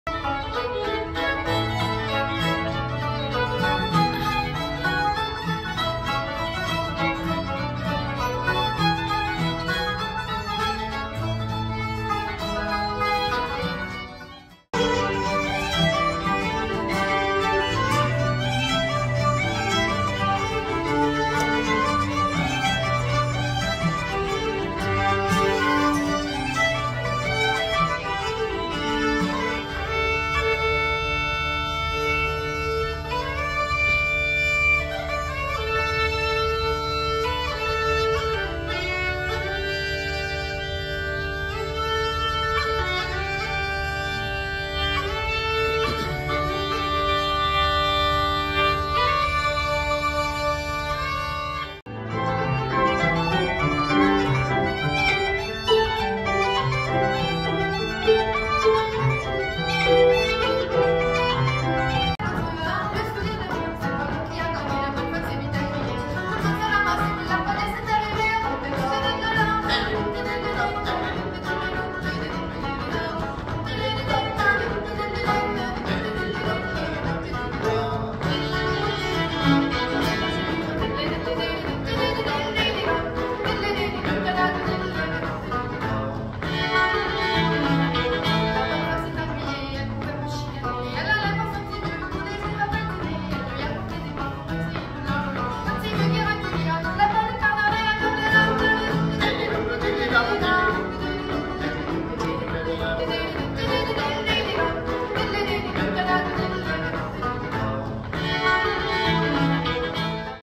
Grosse Isle Visiting Artist
March 2024 Quad City Arts had Grosse Isle as part of their visiting artist series. We had them join us for a lovely concert in the library! Here are some clips!